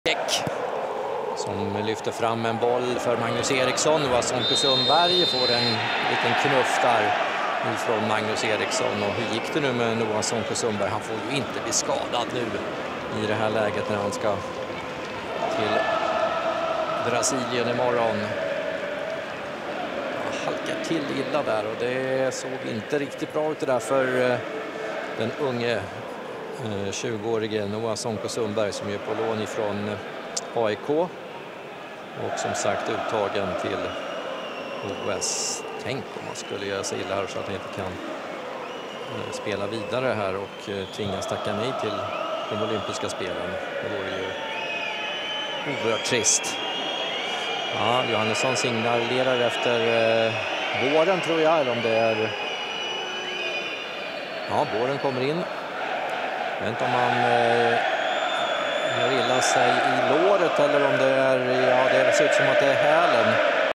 OS-backen Sonko Sundberg skadad inför OS - TV4 Sport
TV4 Sport från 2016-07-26: Noah Sonko Sundberg tvingades bryta tidigt i matchen mot Djurgården, han vred till fotleden dagen före OS-truppen reser till Brasilien.